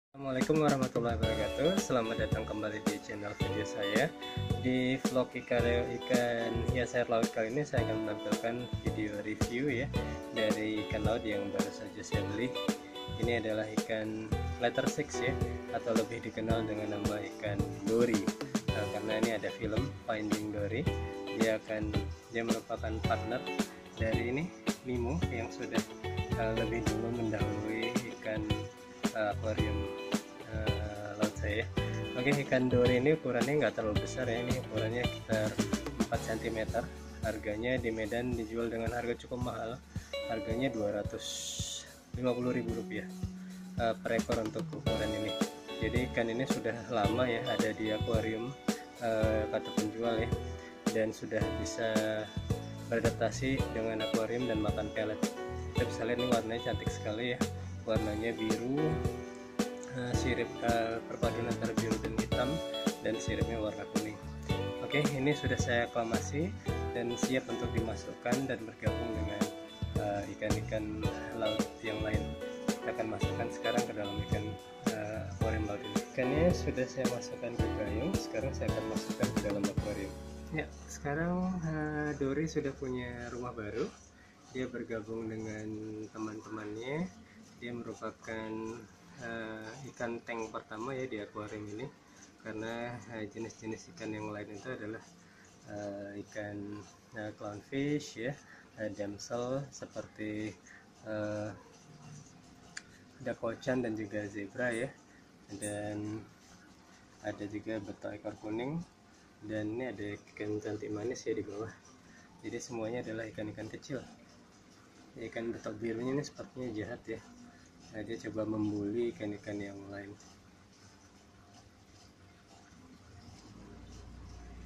IKAN HIAS AIR LAUT : DORY
Review Ikan Dorry/ Leter Six/ Blue Tang di Aquarium Laut Mini.